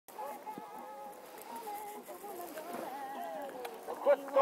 Rodeo540/flatspin540
Ok, one year after my first video this is how my rodeo has become, or actually more a flatspin, but who cares? ;) if you haven't checked my other video out, please go a head.!

come up a little bit short on this jump though.. but the only shot i have of it!